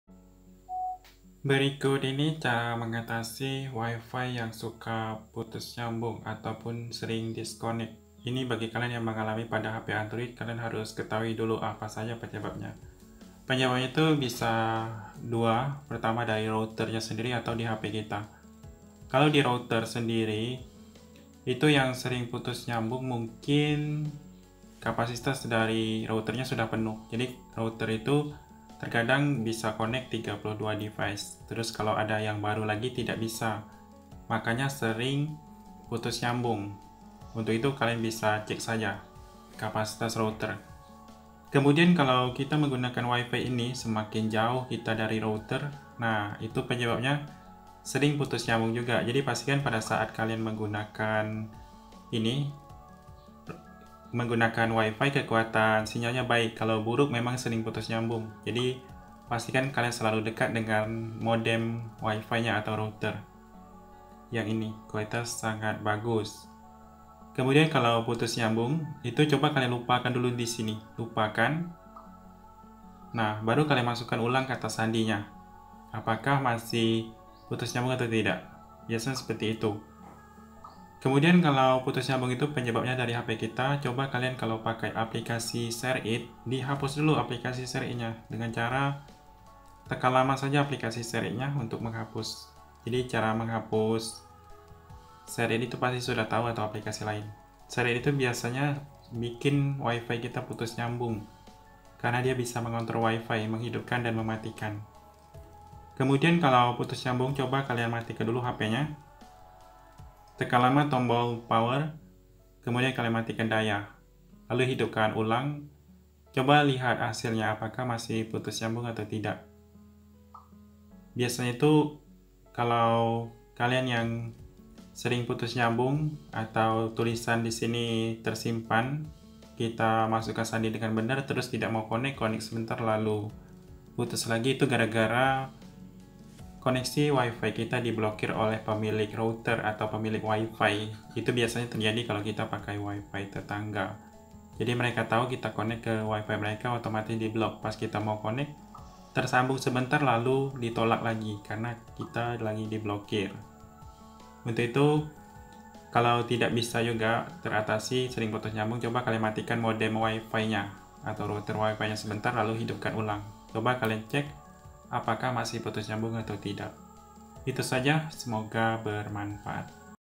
Cara mengatasi wifi disconnect sendiri di android
Bagaimana untuk membaiki wifi memutuskan sambungan sendiri pada android